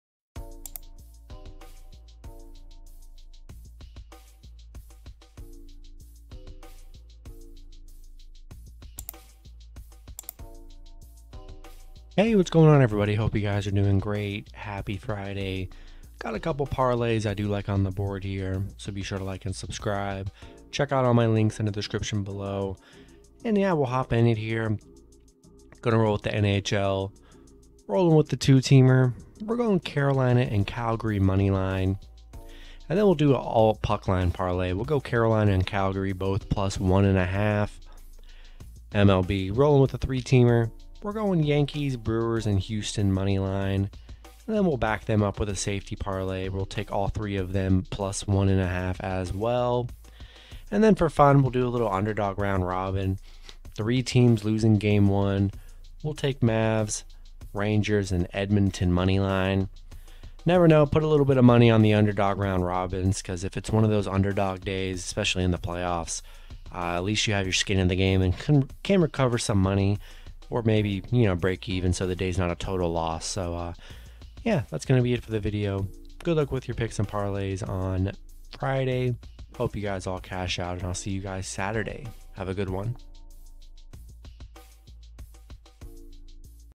Free NBA, NHL and MLB Parlay Options 5/20/22
Here are my Free NBA, NHL, and MLB Parlay Options/ Teasers for today!

🤑2. Bovada

Free NBA Parlay
Free NHL Parlay
Free NCAAB Parlay
Free MLB Parlay
Free NFL Parlay
Free NCAAF Parlay

✔️ Parlays Today:
NHL
Carolina ML
Calgary ML

Carolina +1.5
Calgary +1.5

MLB
Yankees ML
Brewers ML
Houston ML

Yankees +1.5
Brewers +1.5
Houston +1.5

Underdog Round Robin
Mavs ML
Rangers ML
Edmonton ML